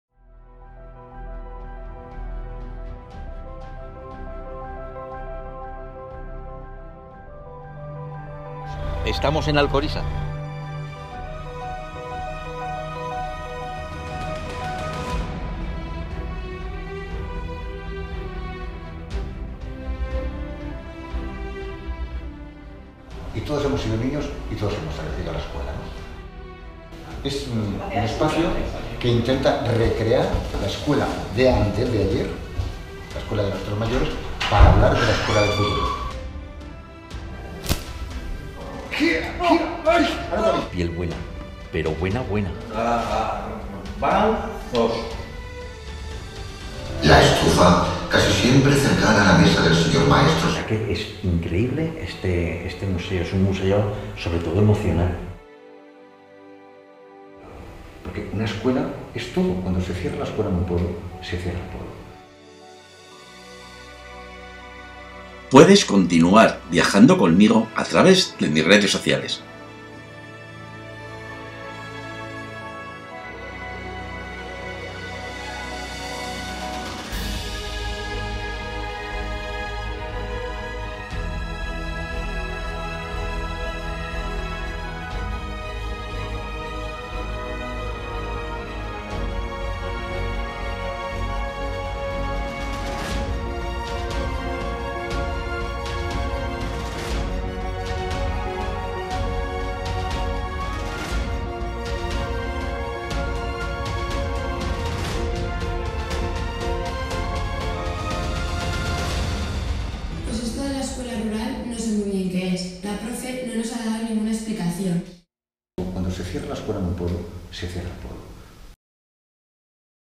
Museo de la Escuela Rural de Teruel en Alcorisa con silla de ruedas
Descubrimos el Museo de la Escuela Rural de Teruel con silla de ruedas que se encuentra en Alcorisa y en el que disfruté gracias a su accesibilidad.

Alcorisa se encuentra en la Comarca del Bajo Aragón de Teruel y nos hemos acercado para conocer el pueblo y su Museo de la Escuela Rural de Teruel, un proyecto precioso realizado por el futuro de esta sociedad, que son los niños, y aunando esfuerzos de muchas personas que han hecho posible algo realmente bonito y accesible con silla de ruedas y para muchas personas.

Alcorisa tiene varias plazas de aparcamiento reservadas a personas con movilidad reducida y una de ellas se encuentra en la entrada al museo al que se accede sin barreras arquitectónicas, a través de un ascensor para subir a la primera planta y tiene baños adaptados.

El Museo lo impulsa la Asociación de Amigos de la Escuela con mucho esfuerzo e ilusión y se movilizaron 100 pueblos que donaron material y 3.000 niños que los restauraron. Los visitantes pueden donar objetos escolares, convirtiendo al museo en un organismo vivo que crece y mejora constantemente.

Como dice uno de sus fundadores, Salvador Berlanga Quintero es "el espacio que intenta recrear la escuela de antes de ayer para hablar de la escuela del futuro".

Gracias por acompañarme.